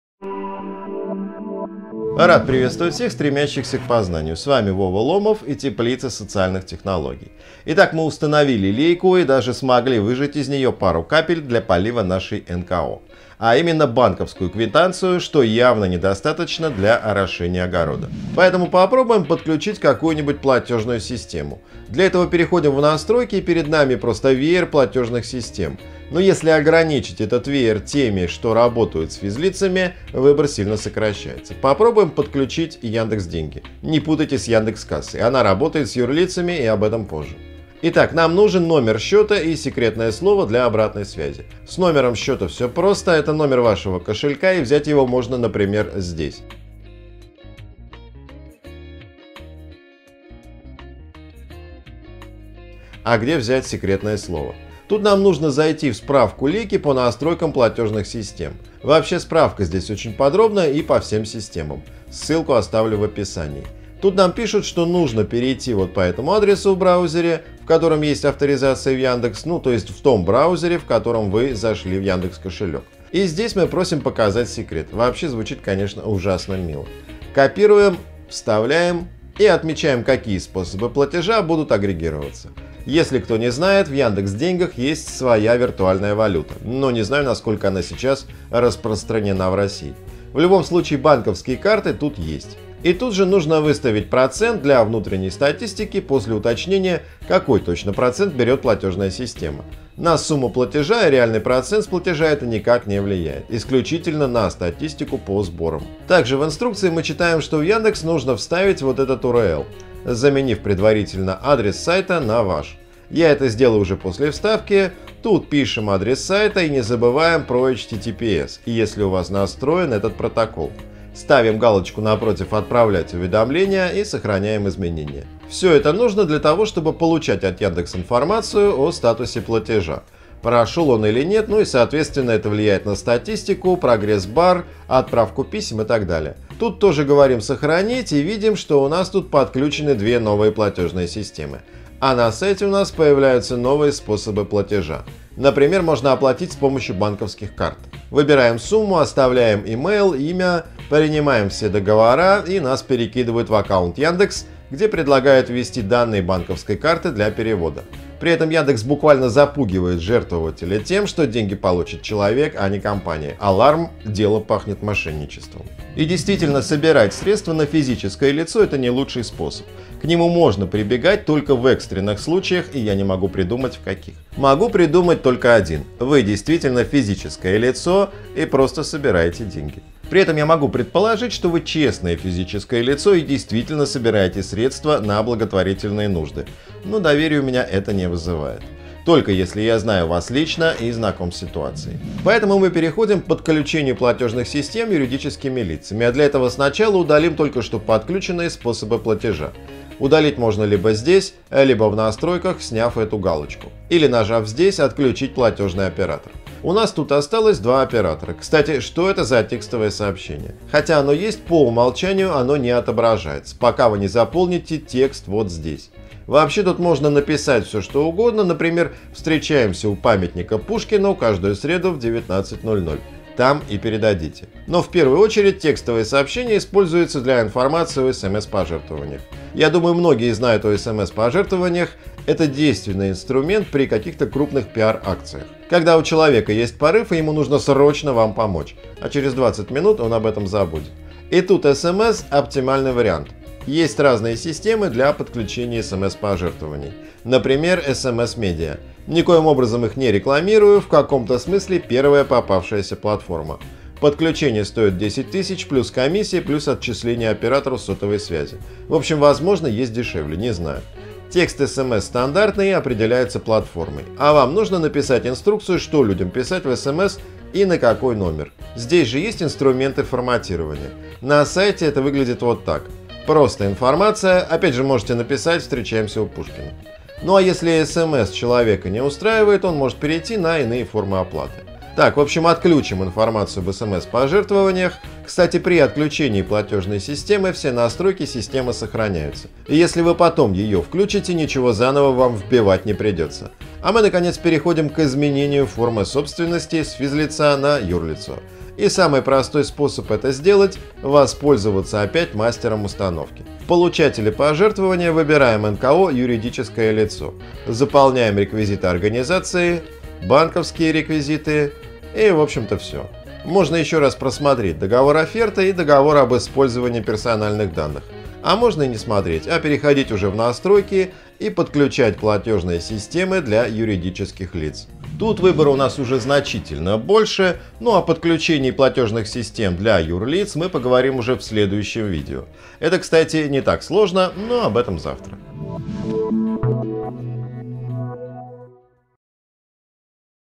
Онлайн плагин Лейка #3: подключение платежных систем для физических лиц
Плагин для WordPress Лейка — одна из главных и наиболее известных разработок Теплицы социальных технологий. Бесплатный плагин Лейка позволяет собирать пожертвования на вашем сайте посредством множества платежных систем, управлять платежами, работать с донорами и получать детальную отчетность.